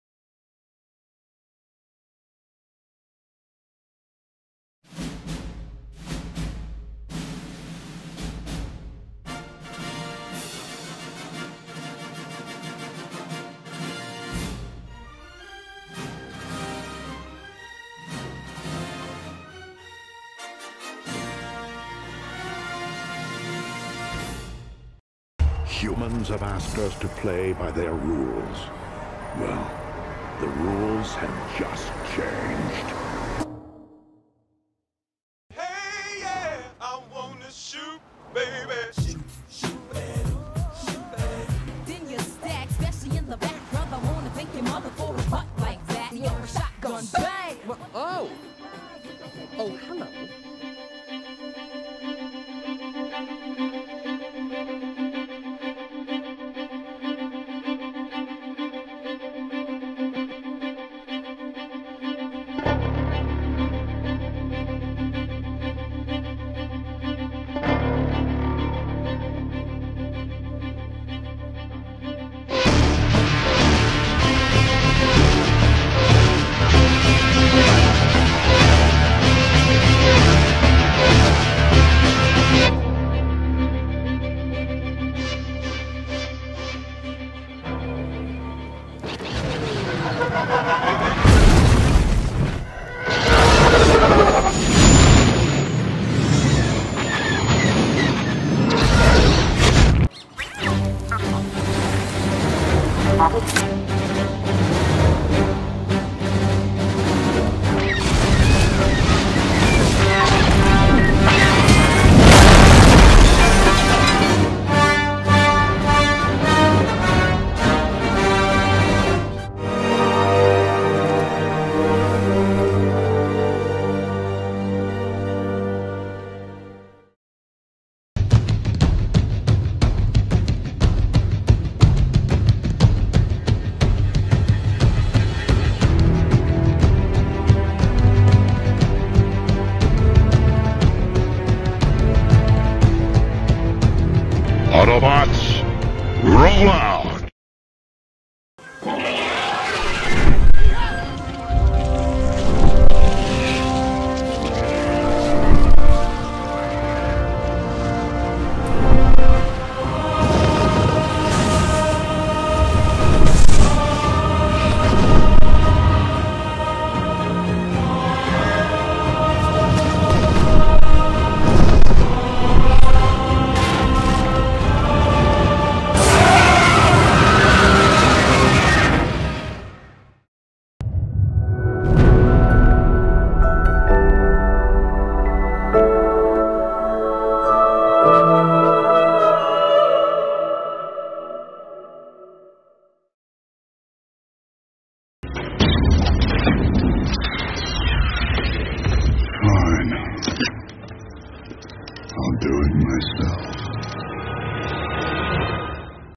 Realms of Fiction - Curtain Raiser by Film Making Club, BITS Pilani
Humans have asked us to play by their rules, well the rules have just changed. Breaking the shackles of fiction these legends paid a visit to BITS Pilani this Oasis.

"Whenever, we go to colleges, they ask us to rate it. We can see that it is amateurish work, but we won't have the heart to say it on their face. So we say that it is good. This is the first time I am seeing professional work.The editing is good, the execution even better. Keep it up." - SS Rajamouli, Director of Bahubali

Directed by: Darshit Khajanchi

Editing: Soorya Rethinasamy, Jays Jose Thomas, Yash Gupta, Achintya Tiwari

Colorist: Mayank Mundada

Storyboarding: Aditya Pai

Camera and Sound: Mridul Poddar, Saurabh Jain

The video was a Curtain Raiser to BITS Pilani's Oasis '17 - Realms of Fiction.